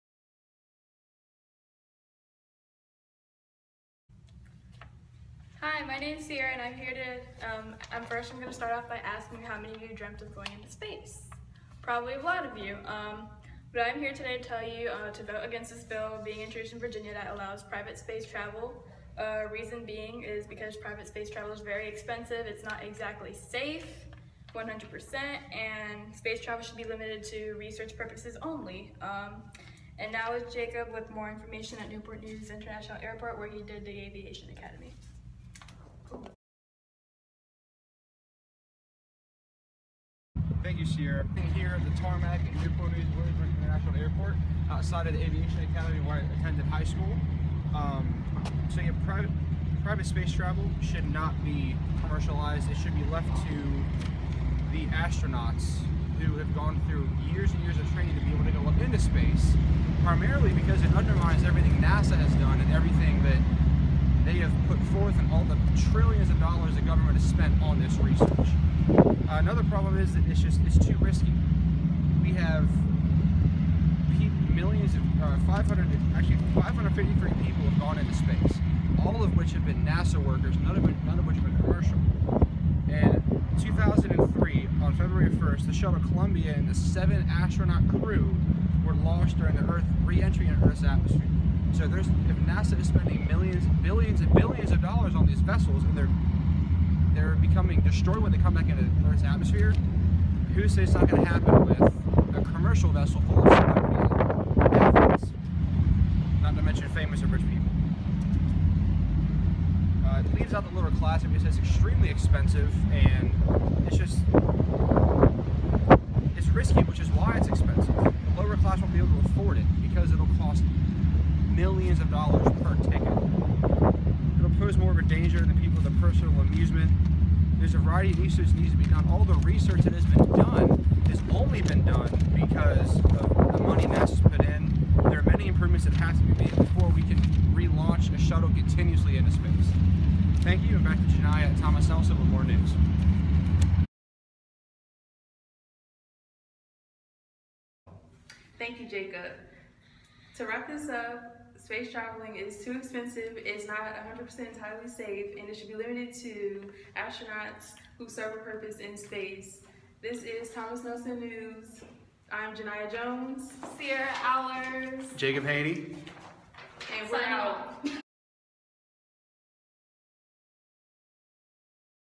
CST Speech: Opposition to Commercial Space Flight
Group Video Project
Hanny,Jones, Alers
Thomas Nelson Community College
Principles of Public Speaking
CST 100-24H
Professor Claudette White